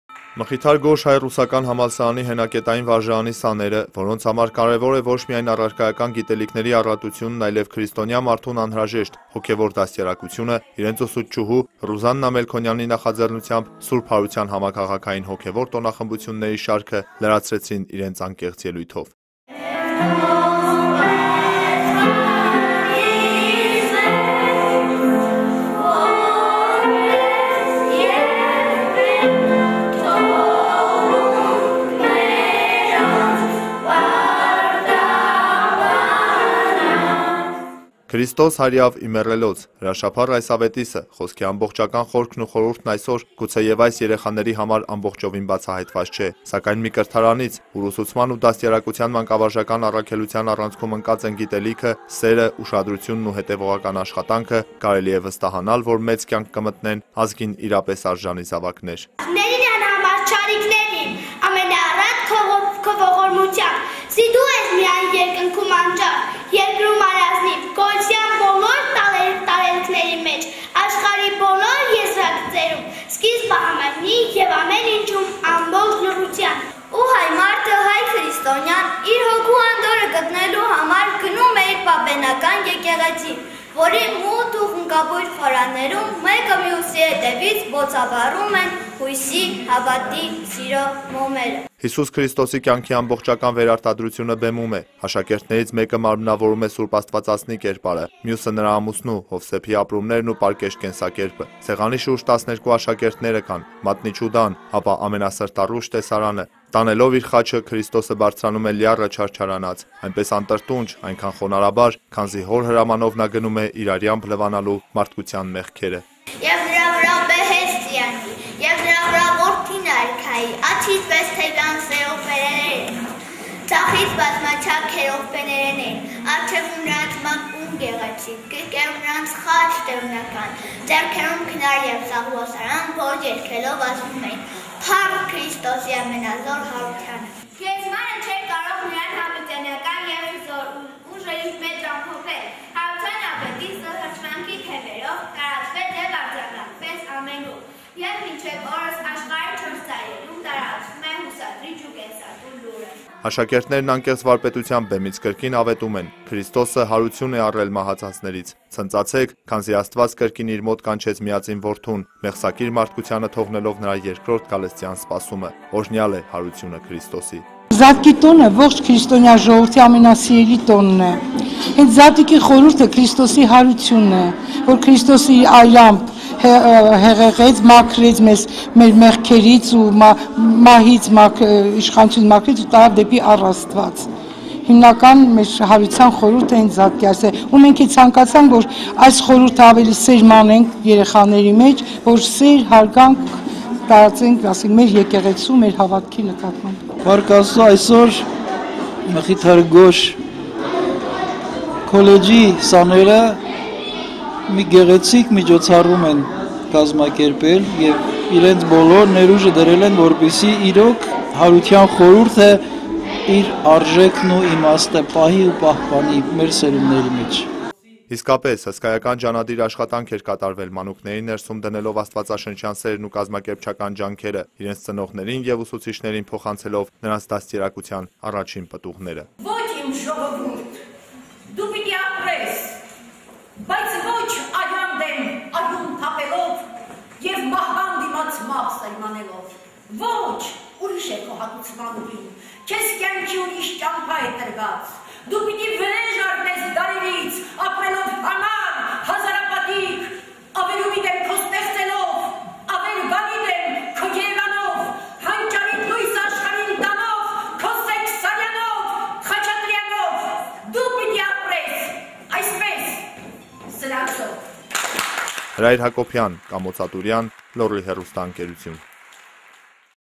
Սուրբ հարության համաքաղաքային հոգևոր տոնախմբությունների շարքը համալրվեց ևս մեկ միջոցառմամբ
Մխիթար Գոշ հայ-ռուսական համալսարանի հենակետային վարժարանի սաները, որոնց համար կարևոր է ոչ միայն առարկայական գիտելիքների առատությունն այլև քրստոնյա մարդուն անհրաժեշտ հոգևոր դաստիարակությունը, իրենց ուսուցչուհու Ռուզաննա Մելքոնյանի նախաձեռնությամբ Սուրբ հարության համաքաղաքային հոգևոր տոնախմբությունների շարքը լրացրեցին իրենց անկեղծ ելույթով: